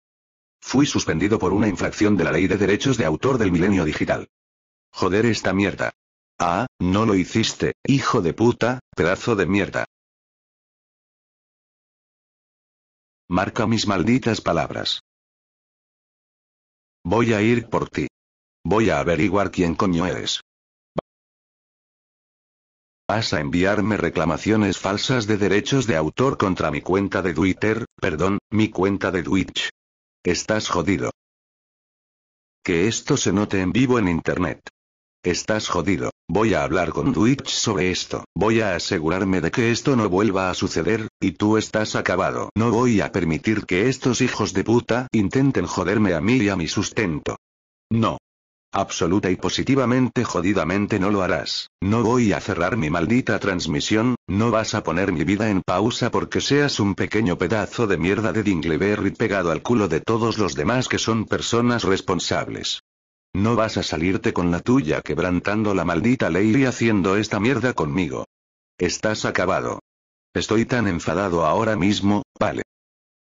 DSP is coming for you [SPANISH DUB]
Fui suspendido por una infracción de la Ley de Derechos de Autor del Milenio Digital. Joder esta mierda. Ah, no lo hiciste, hijo de puta, pedazo de mierda. Marca mis malditas palabras: ¡Voy a ir por ti! Voy a averiguar quién coño eres. Vas a enviarme reclamaciones falsas de derechos de autor contra mi cuenta de Twitter, perdón, mi cuenta de Twitch. Estás jodido. Que esto se note en vivo en internet. Estás jodido. Voy a hablar con Twitch sobre esto. Voy a asegurarme de que esto no vuelva a suceder, y tú estás acabado. No voy a permitir que estos hijos de puta intenten joderme a mí y a mi sustento. No. Absoluta y positivamente jodidamente no lo harás. No voy a cerrar mi maldita transmisión, no vas a poner mi vida en pausa porque seas un pequeño pedazo de mierda de dingleberry pegado al culo de todos los demás que son personas responsables. No vas a salirte con la tuya quebrantando la maldita ley y haciendo esta mierda conmigo. ¡Estás acabado! Estoy tan enfadado ahora mismo, vale.